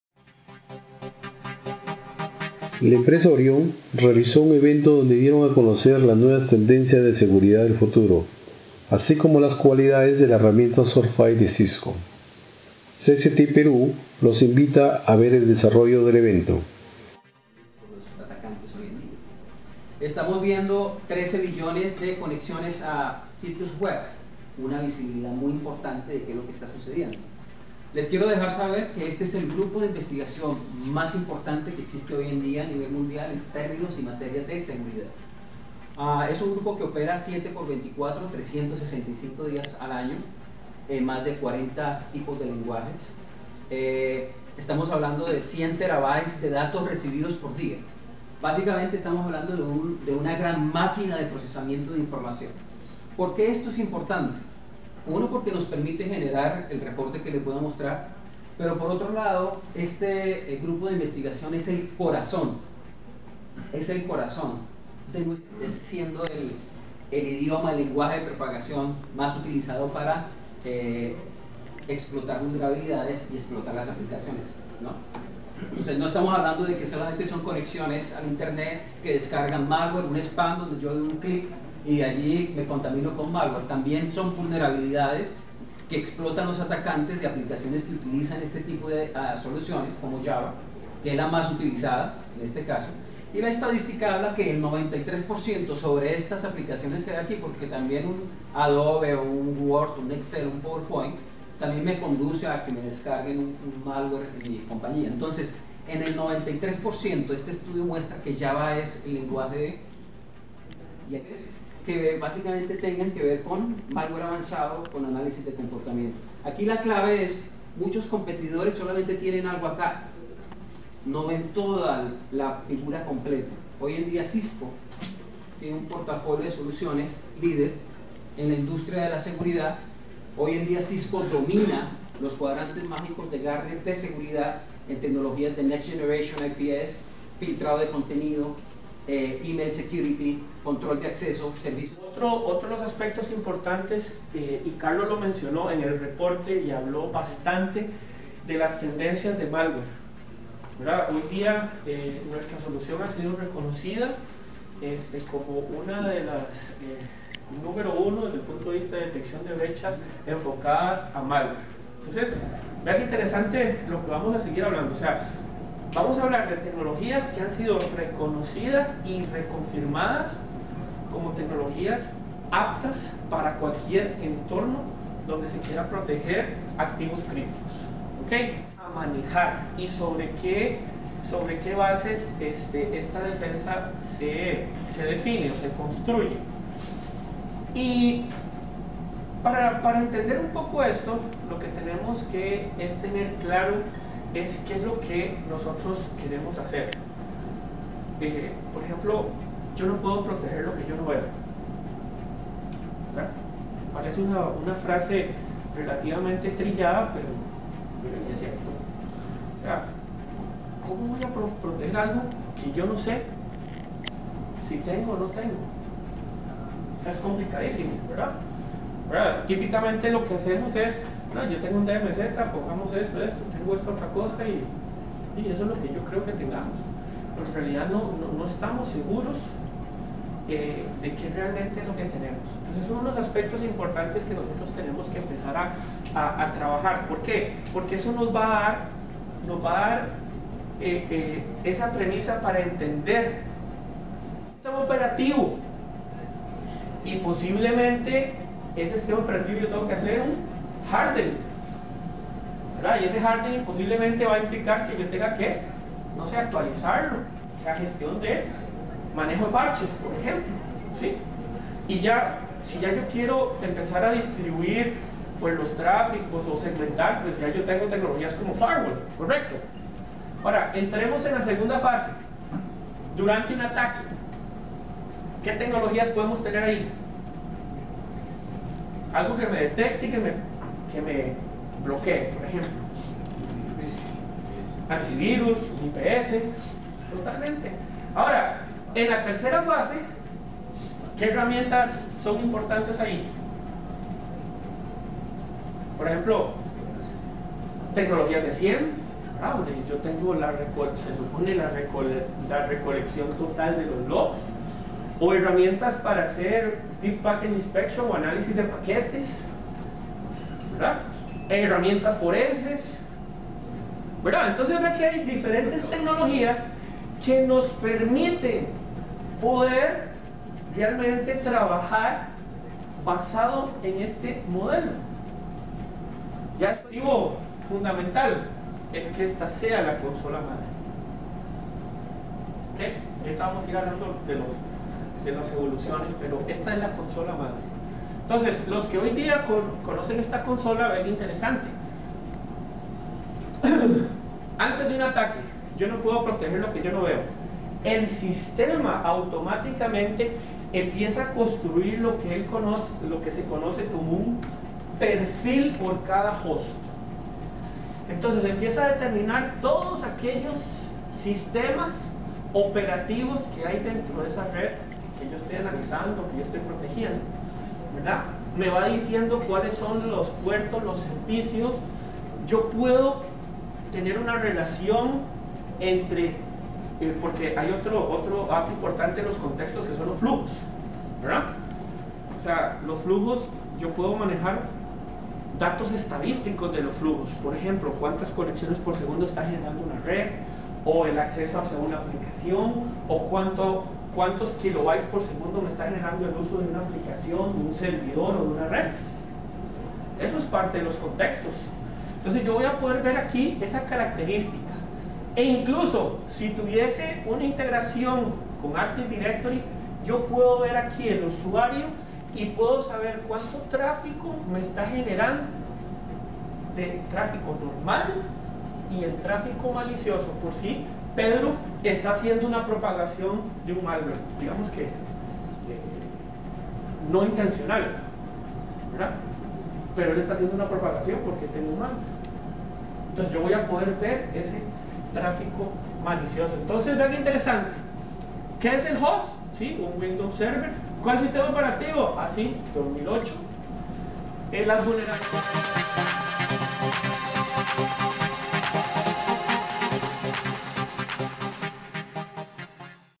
Orion realizo el evento “Seguridad del Futuro”.
CSTIC PERU, estuvo presente en el evento “Seguridad del Futuro” que realizo la empresa Orion.